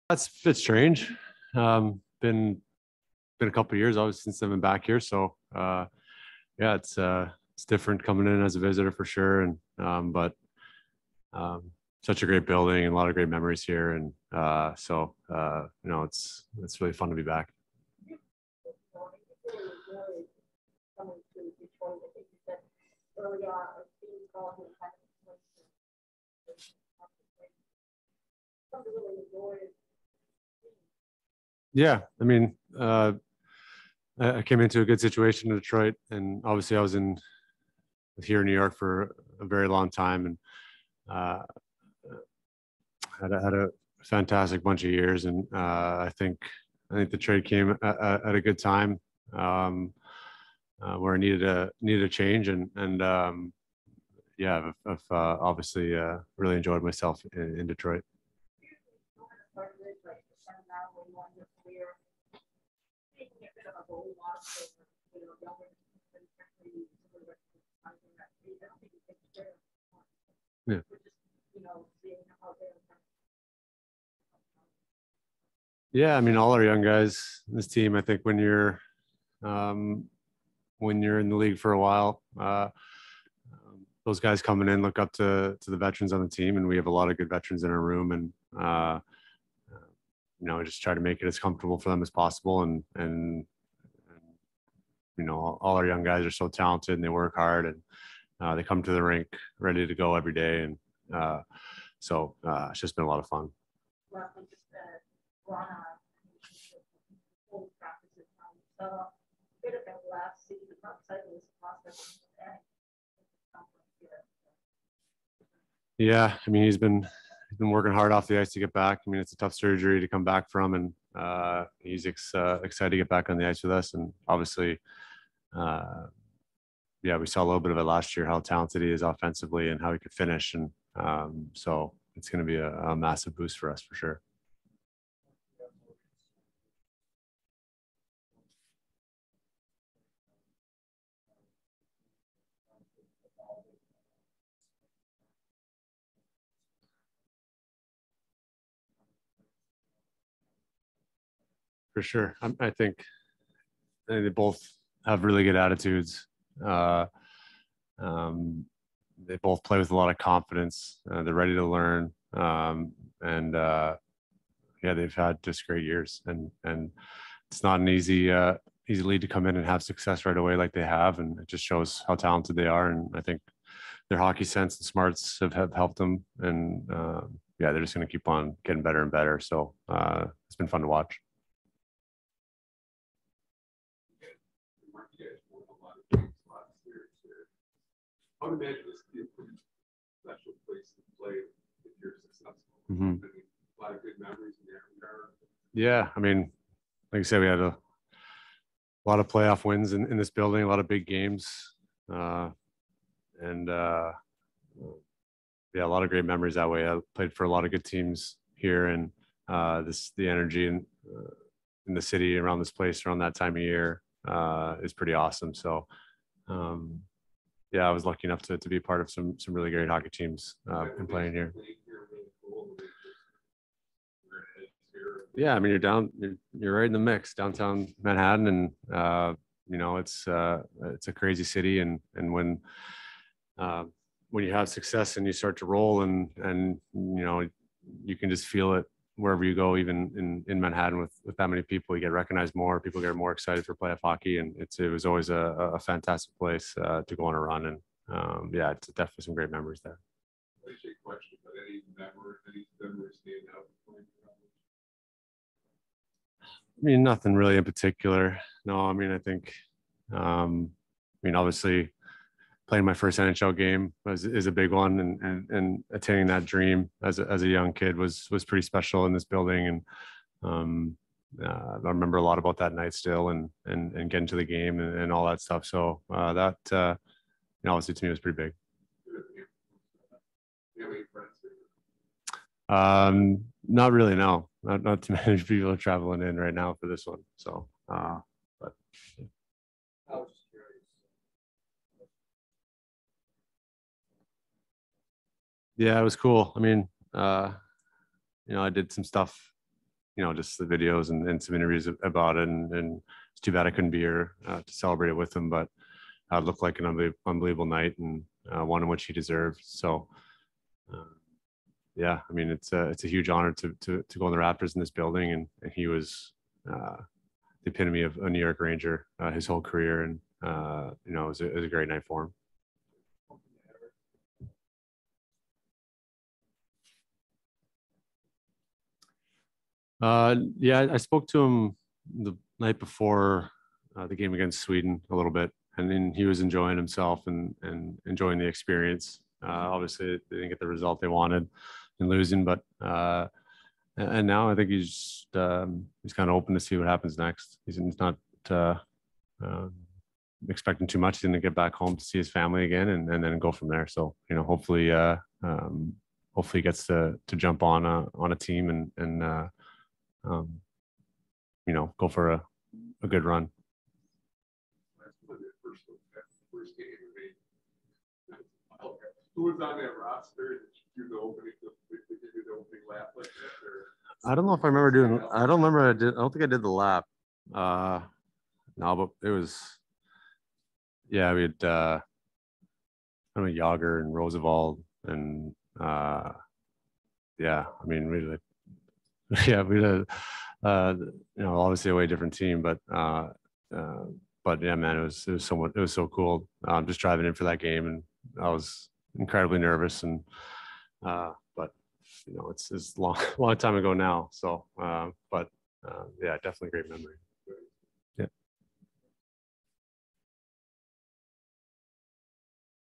Marc Staal | Detroit Red Wings Morning Skate | 2/17/22 NYR
Marc Staal speaks with the media following Red Wings morning skate in New York on Thursday, February 17th.

Presented by World Wide Technologies